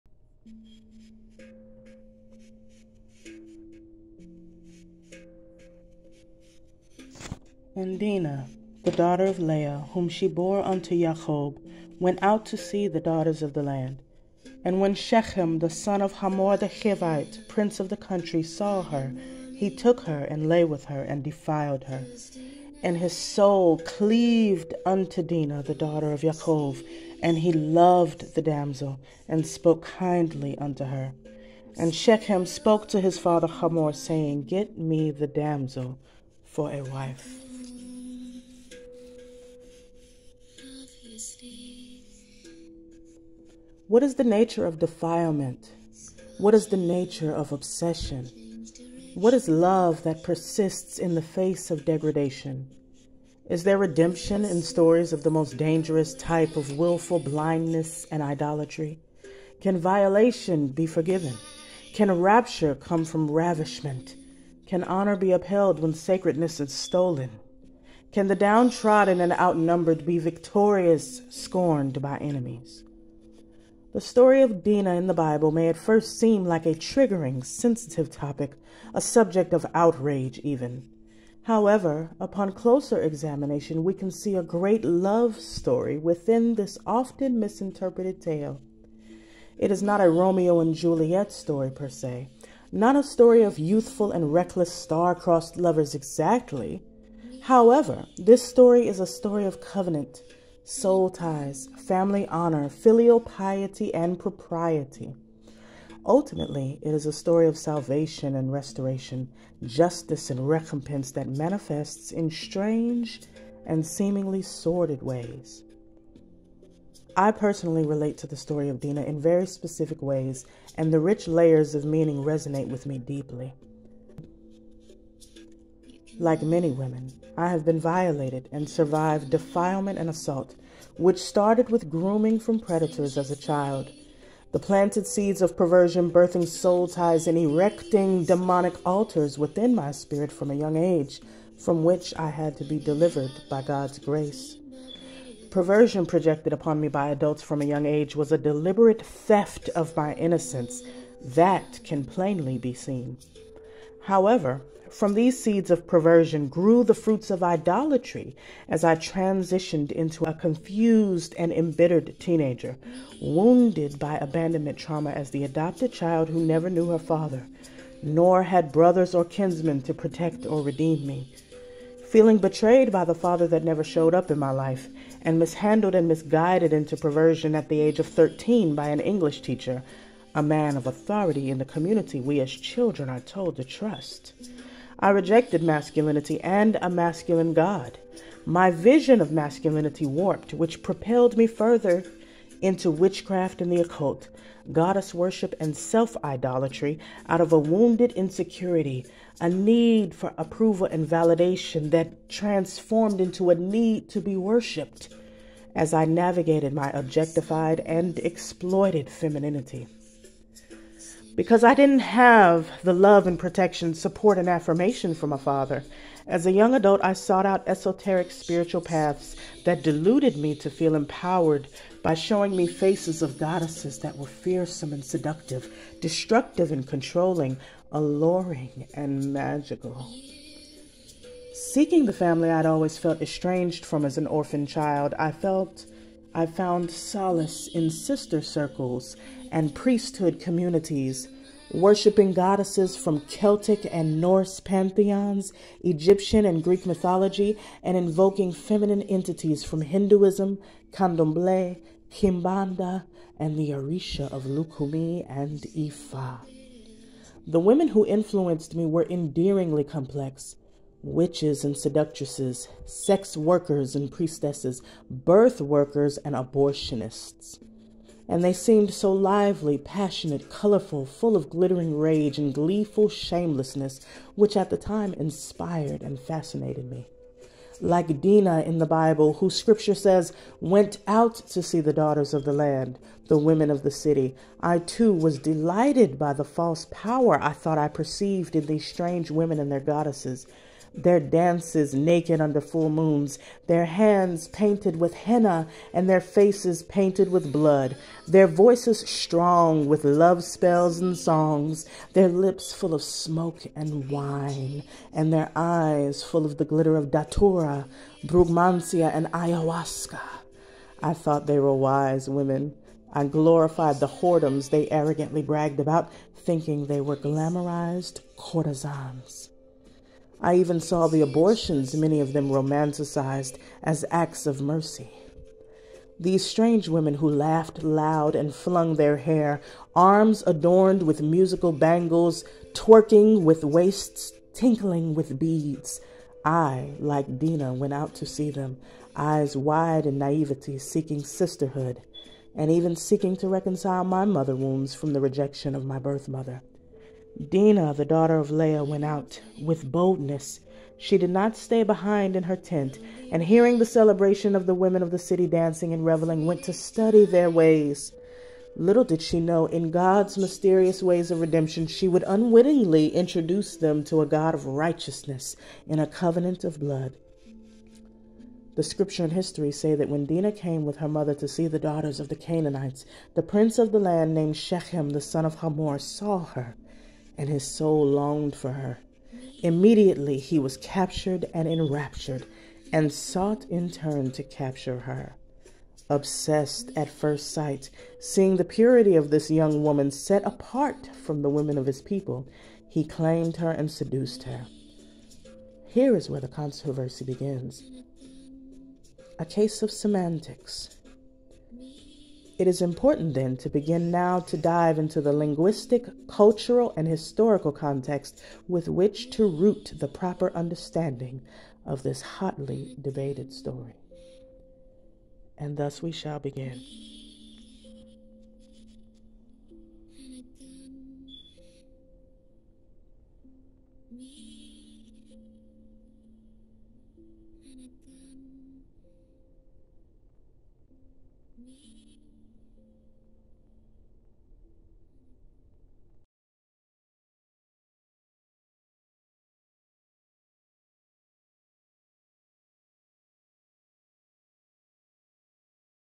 Part1 My Testimony in the Story of Dinah: Redemption from Ravishment
What is the nature of defilement? What is the nature of obsession? What is love that persists in the face of degradation? Is there redemption in stories of the most dangerous type of willful blindness and idolatry? Can violation be forgiven? Can rapture come from ravishment? Can honor be upheld when sacredness is stolen? Can the downtrodden and outnumbered be victorious scorned by enemies?

The story of Dinah may at first seem like a triggering, sensitive topic, a subject of outrage, even. However, upon closer examination, we can see a great love story within this often misinterpreted tale. It is not a Romeo + Juliet story per se, not a story of youthful and reckless star crossed lovers exactly; however, this story is a story of covenant, soul ties, family honor, filial piety, and propriety. Ultimately, it is a story of salvation and restoration, justice and recompense that manifests in strange and seemingly sordid ways.

By God's Grace, we are walking testimonies! Sharing part of my testimony of "going out among the daughters of the land" and why the story of Dinah resonates so deeply with me. A victim of childhood predatory grooming and assault, as well as physical & sexual abuse in my previous marriage, I have also been forcibly violated while crying out and I am a survivor of several forms of defilement and degradation, able to relate to many women in different experiences, demonic covenants, and soulties . In this part of my story, I relate to Dinah's fascination with the Pagan women of the Canaanite land in which her family sojourned, and how her "going out" among them led to greater tragedy, as it did in my own life in a particularly nuanced way that reflects and parallels Dinah's narrative. As this controversial story is often debated as a terrifyingly UNJUST occasion in which many believe God "allowed" the violation of an unprotected woman, I will offer an alternate perspective as I will continue this with a Part 2 in which the Hebrew language is illuminated, and making an irrefutable distinction between forced violation and the nature of seduction and coercion implemented by Shechem to rob DInah of her honor. Was she defiled? Definitely. Whether she was forcibly attacked is another matter altogether-- nonetheless, this is certainly a story of Soul Tie of lust, obsession, and dishonor in which a daughter of Israel was "turned out" and made common as a Harlot, as unfortunately many of our community's young women have...

Dinah, daughter of Leah, went out– with boldness, she did not stay behind in her tent and, hearing the celebration of the women of the city dancing and reveling, went to study their ways. Little did she know, in God’s mysterious ways of redemption, she would unwittingly introduce them to a God of Righteousness in a Covenant of blood.

The scripture and histories say that when Dinah came with her mother to see the daughters of the Canaanites, the price of the land named Shechem, son of Hamor, saw her and his soul longed for her. Immediately, he was captured and enraptured– and sought in turn to capture her. Obsessed at first sight, seeing the purity of this young women, Set Apart from the women of his people, he claimed her and seduced her. Here is where the controversy begins– a case of semantics.

It is important, then, to begin now to dive into the linguistic, cultural, and historical context with which to root the proper understanding of this hotly debated story.

Continue to Part 2 for more.